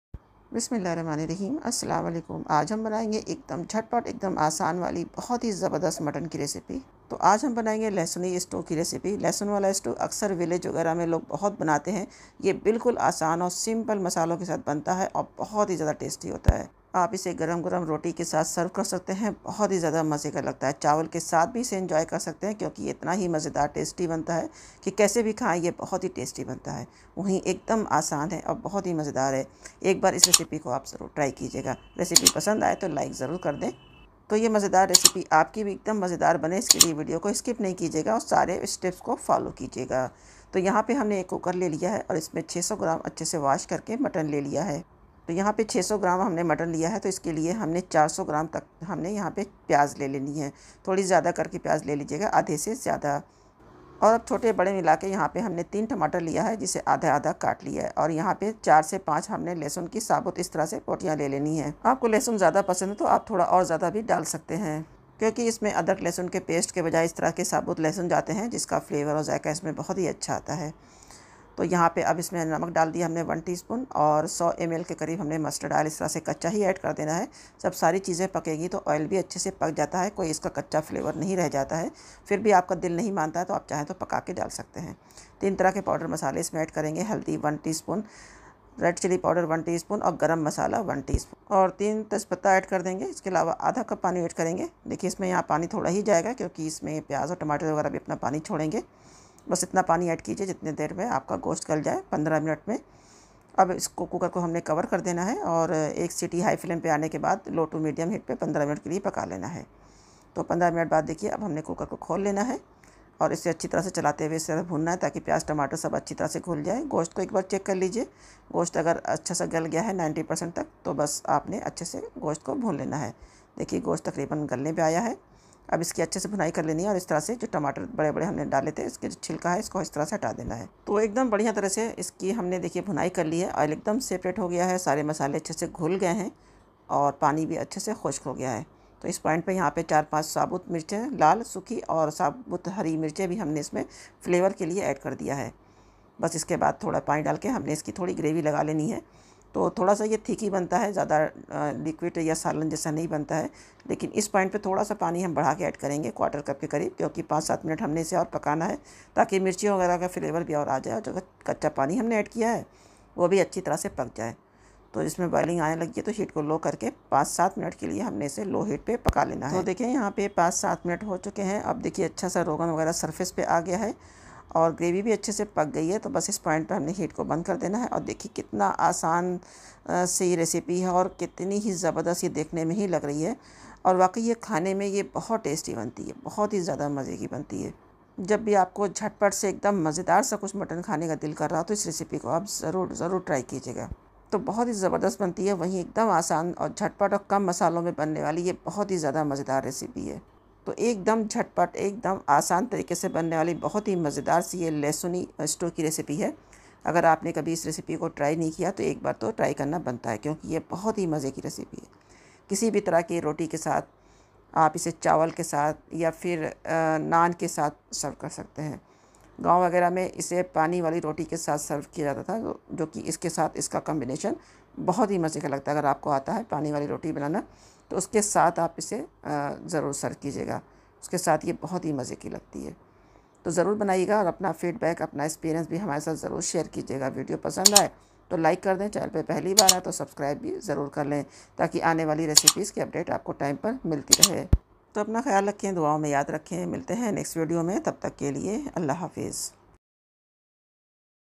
Mutton Stew/Lahsuni Stew Recipe ❤️ Village Style Quick Easy Very Delicious Mutton Stew Recipe ❤️
🎯 Ingredients:-

Onions 300-400 gm
Tomatoes 3 medium sized
Whole garlic 4-5